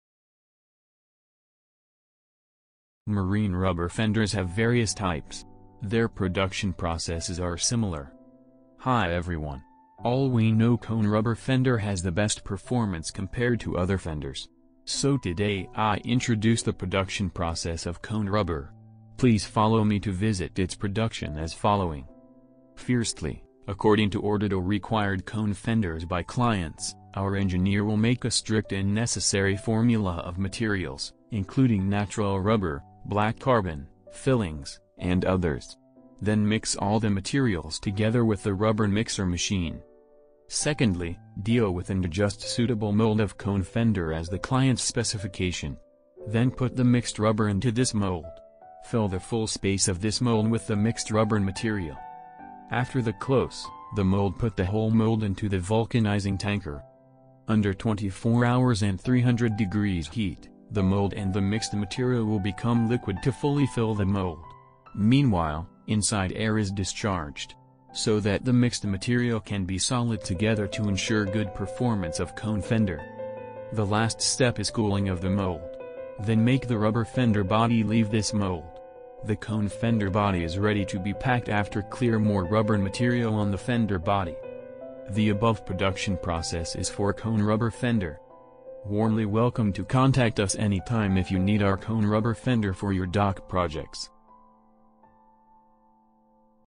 How to produce cone rubber fender?
Marine rubber fenders have various types. Their production processes are similar.
Hi everyone. All we know cone rubber fender has the best performance compared to other fenders. So today I introduce the production process of cone rubber. Please follow me to visit its production as following.